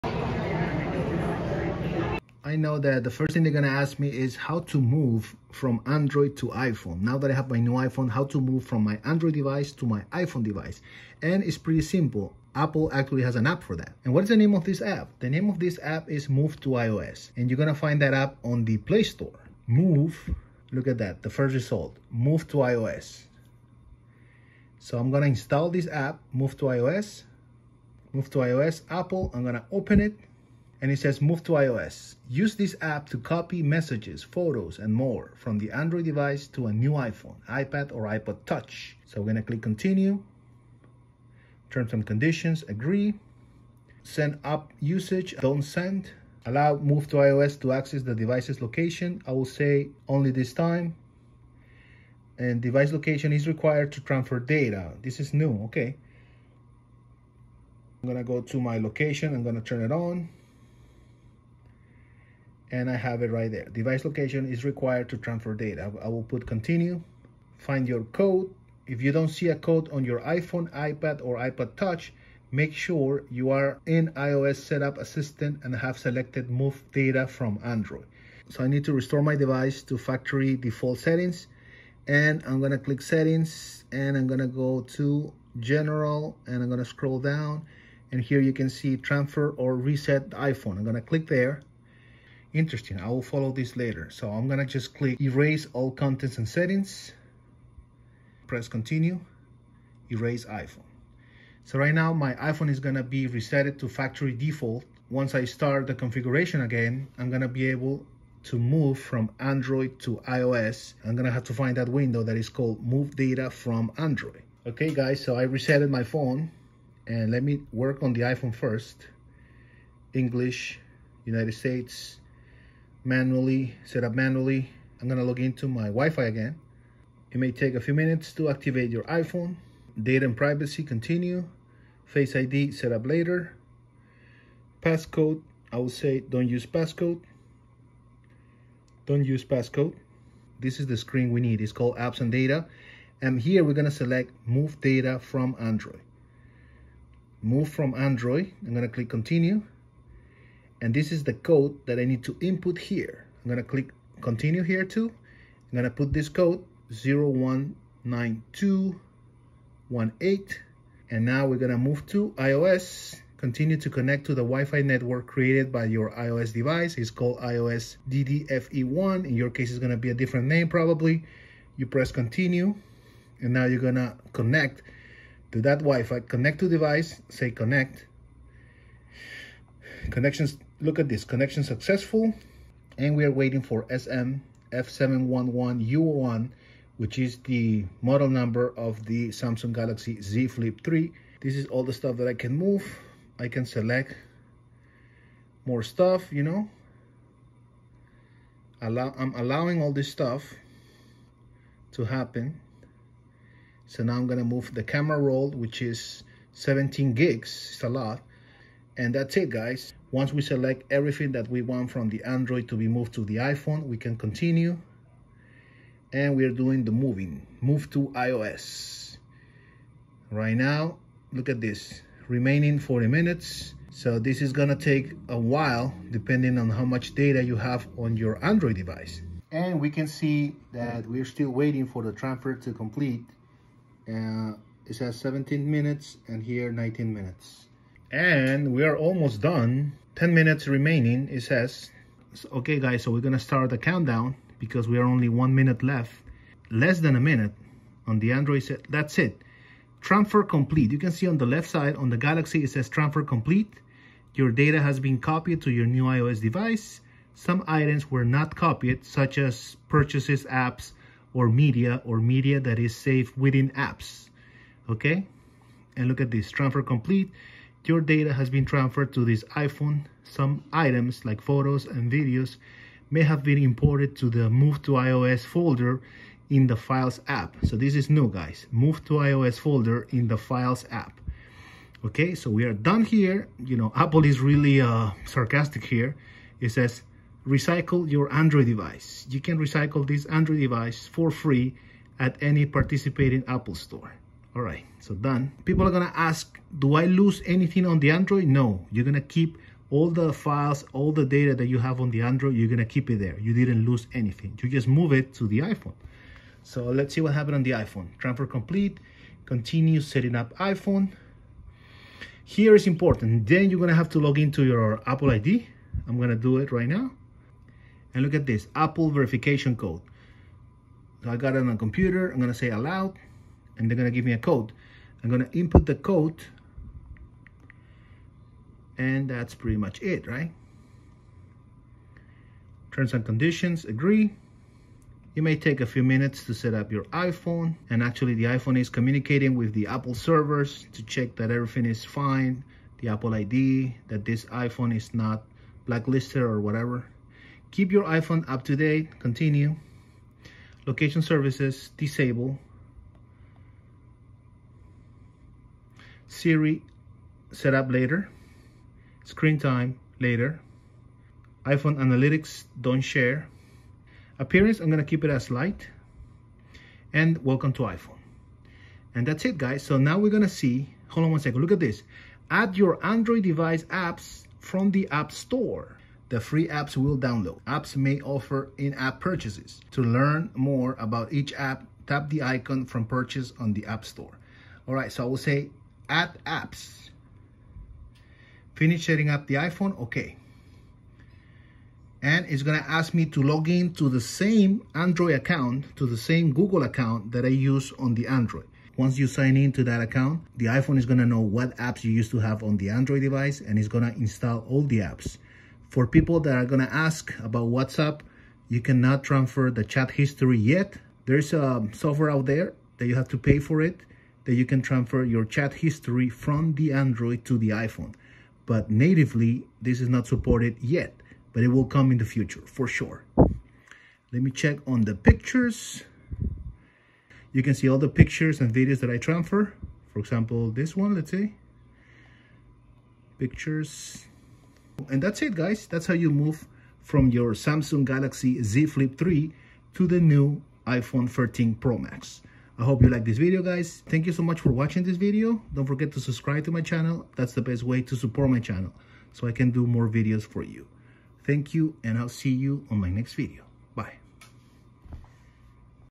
Move your data from Android to iOS with the App Move to iOS 15 Galaxy Z Flip3 to iPhone 13 Pro Max
Move all your data from an Android device to an iOS device with the official Apple App called "Move to iOS" In this video I'm moving all my files from a Samsung Galaxy Z Flip3 to the New iPhone 13 Pro Max with iOS 15.